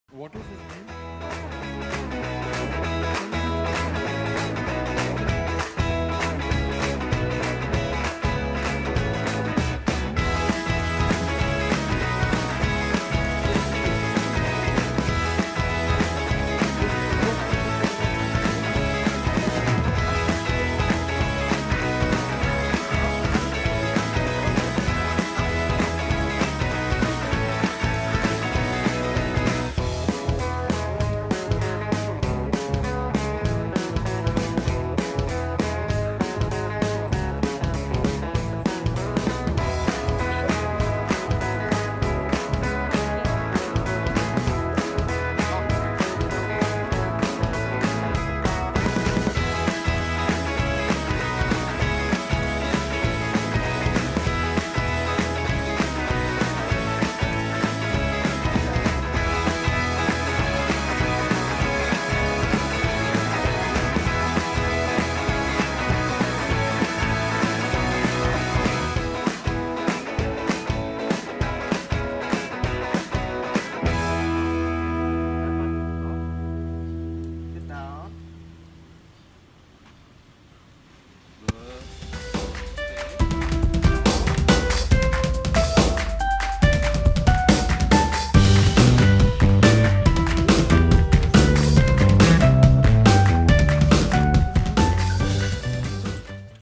FIRST TIME EVER EXPLORING THE NEIGHBORHOOD!
@pepper_golden7 's First experience exploring the neighborhood and met this one nice hooman!

Follow my training sessions in Instagram @pepper_golden7